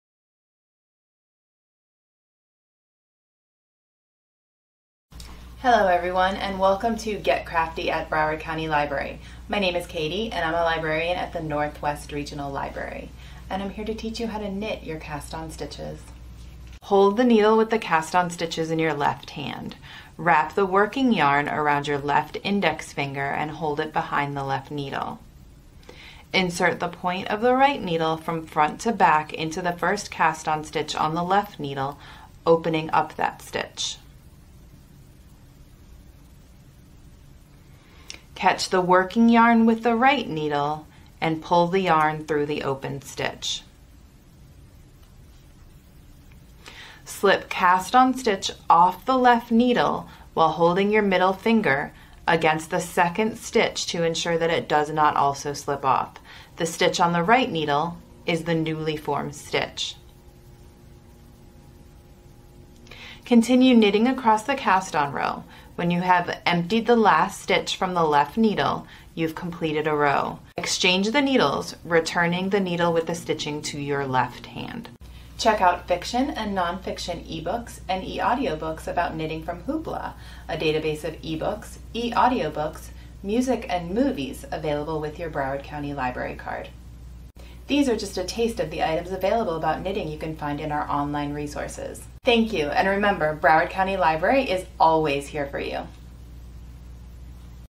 Get Crafty | Knit Stitch Demonstration
Learn the fundamentals of knitting with Katie from Northwest Regional Library in this "Get Crafty" tutorial.